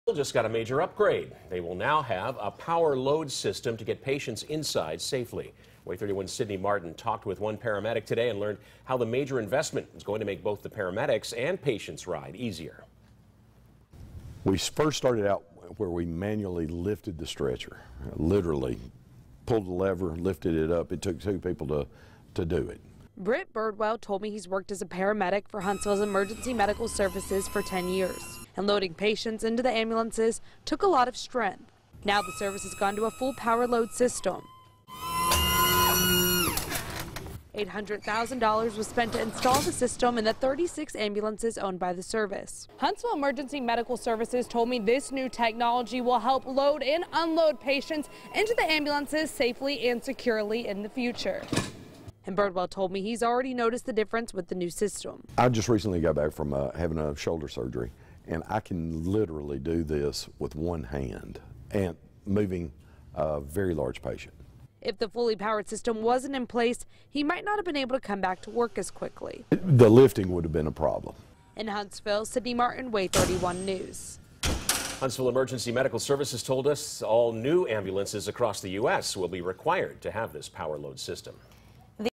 Local Ambulance Company Buys New Stretchers
Sydney Martin talks with a paramedic about the new stretchers that were installed in Huntsville Ambulances.